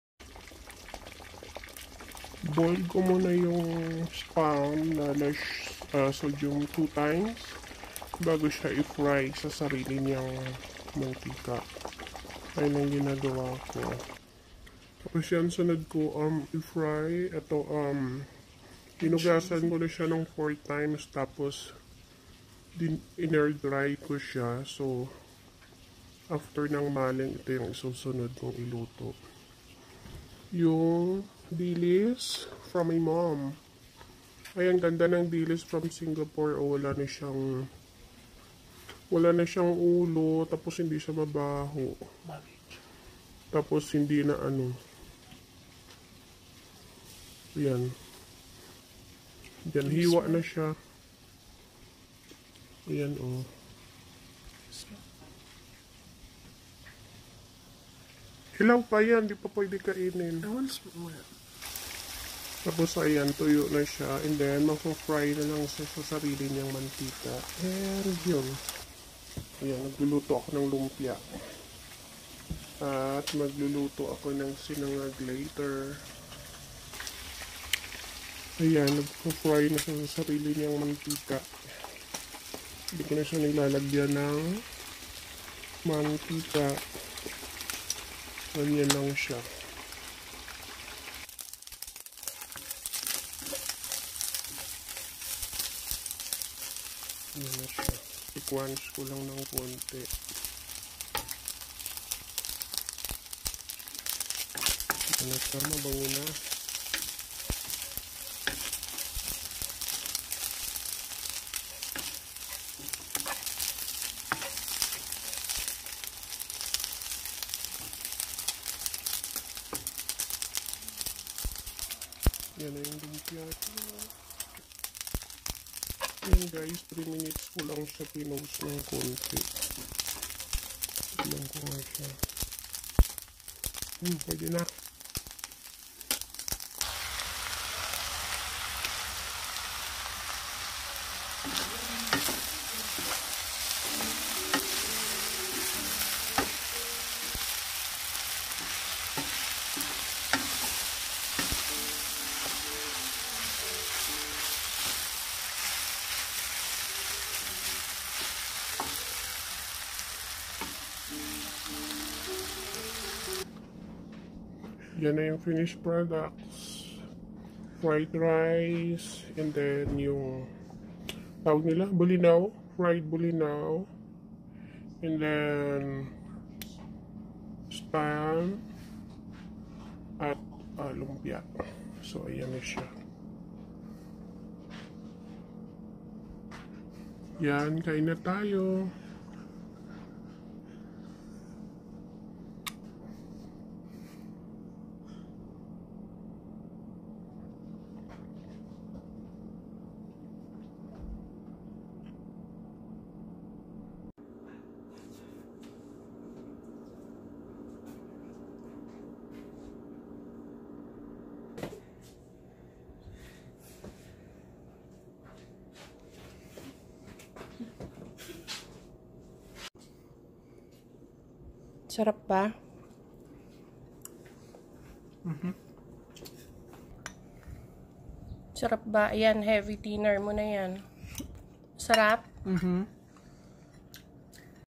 Fried rice+ spam+ egg rolls and fried anchovies/Dinner 🇱🇷
For today's video's I'm going to cook our dinner!